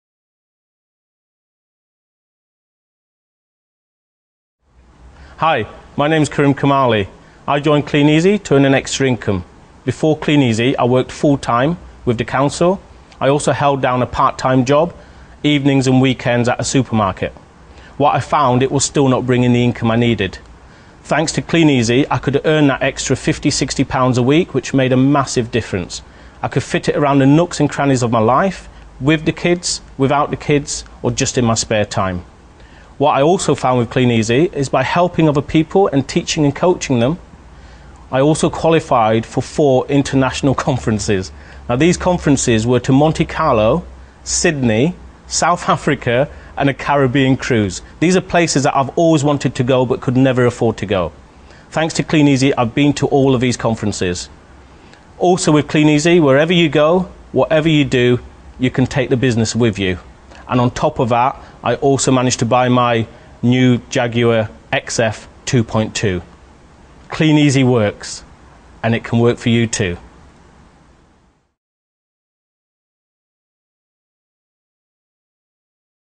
4 It works to give me some of life s luxuries
Karim's story: There were hardly enough working hours available in the week for Karim Karmali to pay the bills in his full-time public sector job in the council and part-time supermarket role. Now he can balance the luxury of family-time with the luxuries of life - including a top-of-the-range Jaguar! If you are not currently a Kleeneze distributor and you would link FREE information on our business, please for a free information pack